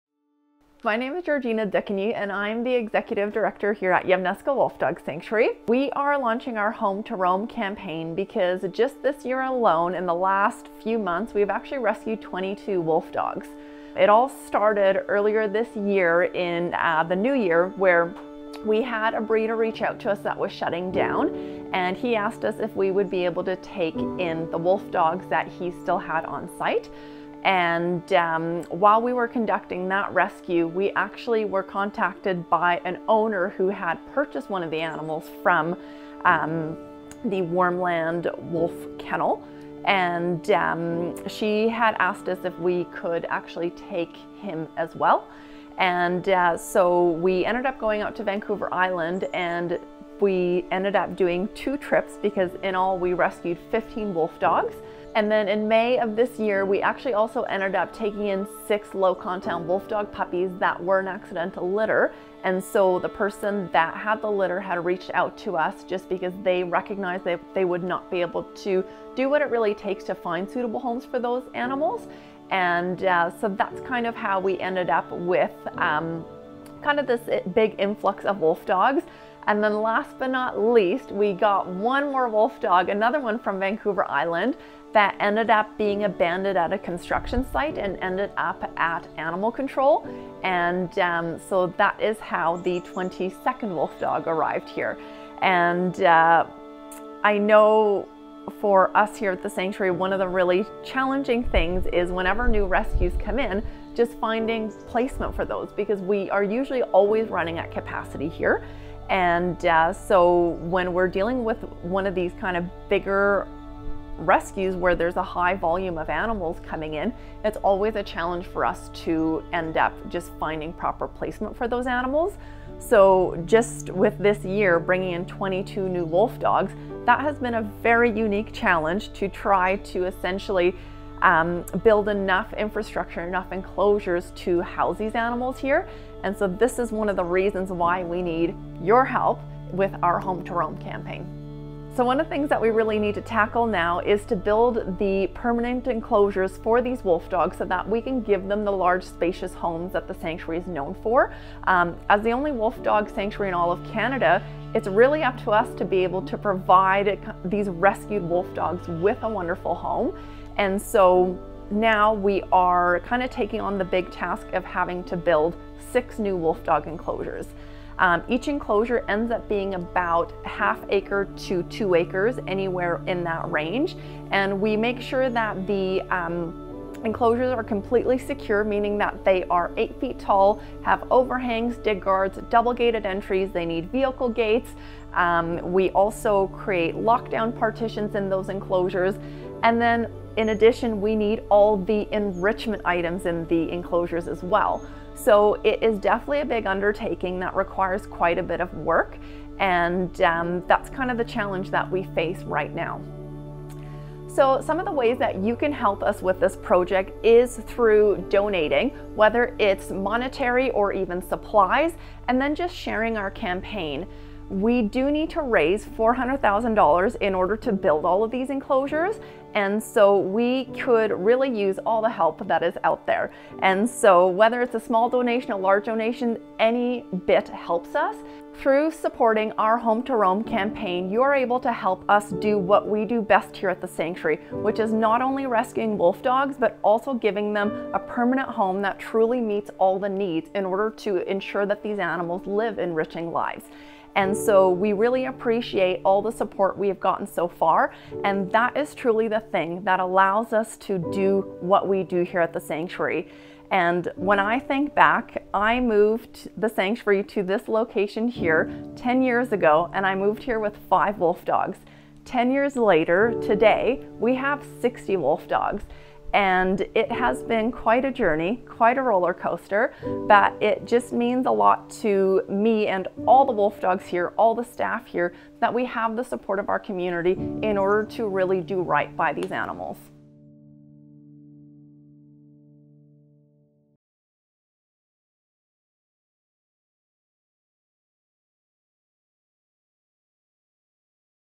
Home to Roam Campaign
We need your support now more than ever. Our Sanctuary is currently operating at full capacity. Just this year, we've rescued 22 wolfdogs, and every day, we receive numerous more calls, emails and messages from people seeking help for wolfdogs in need.

At the beginning of the year, we took in 14 wolfdogs and one coydog from a breeder who was shutting down their operation. In April, we rescued six low-content wolfdog puppies. Most recently, we welcomed Akela, a mid-content wolfdog who was abandoned on the 10th floor of a construction site and spent months in an animal control facility before arriving at our Sanctuary.

Our mission is to provide these rescued wolfdogs with a meaningful and fulfilling life, while also promoting their well-being and understanding within our domestic world.

To continue this vital work, we need your help to build six new, spacious, and secure enclosures that can provide our wolfdogs with the mental and physical stimulation they need while also freeing up space for future rescues. Our goal for this campaign is $400,000. Every contribution, small and large, can make a significant difference in the lives of wolfdogs needing a safe haven...a home to roam.